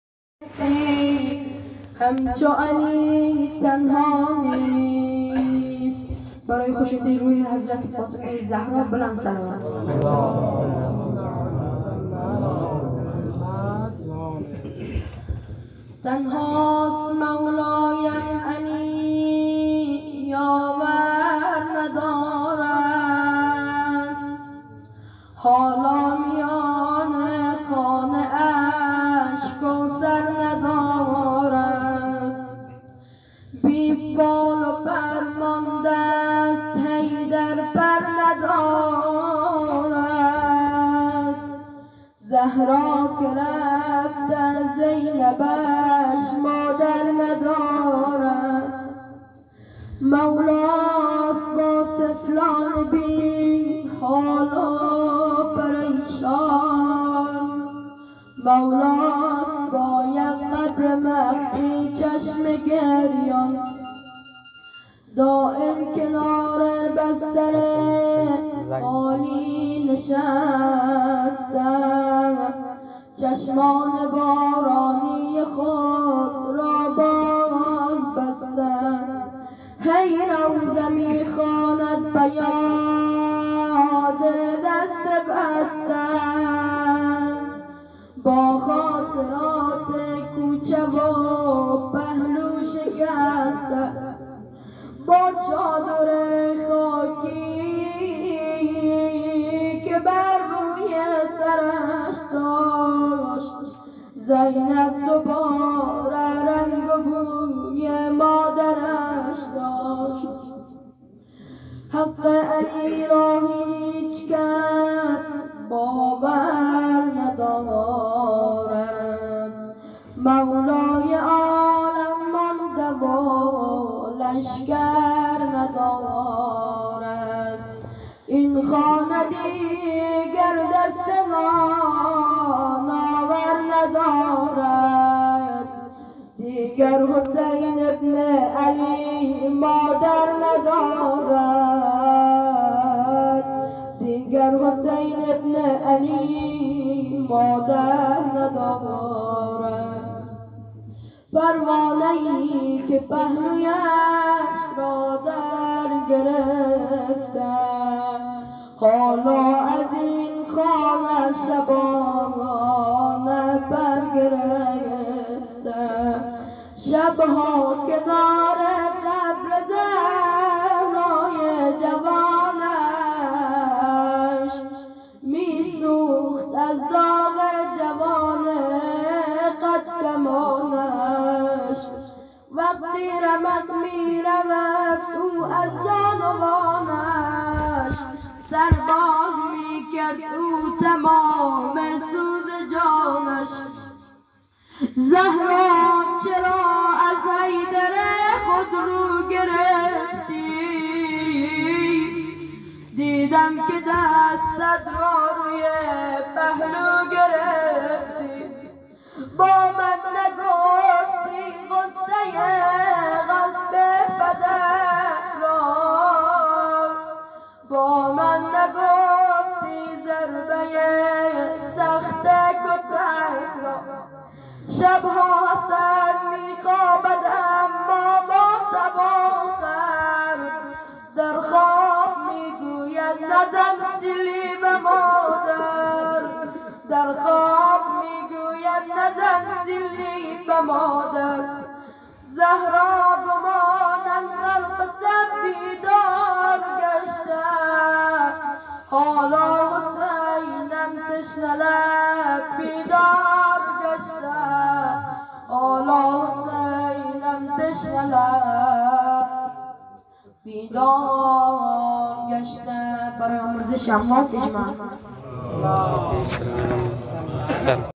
روضه حضرت فاطمه الزهرا
برای دیدن بهترین برنامه های اسلامی چینل یوتیو باران میدیا را سابسکرایب نمائید.
سه شنبه مصادف است با ۳ جمادی الثانی سالروز شهادت دُخت نبوت، همسر ولایت و مادر امامت، اُنسیه حورا حضرت فاطمه الزهرا { س } این مناسبت را به همه ی دوستداران و محبین اهل بیت { ع } تسلیت عرض می نمائیم.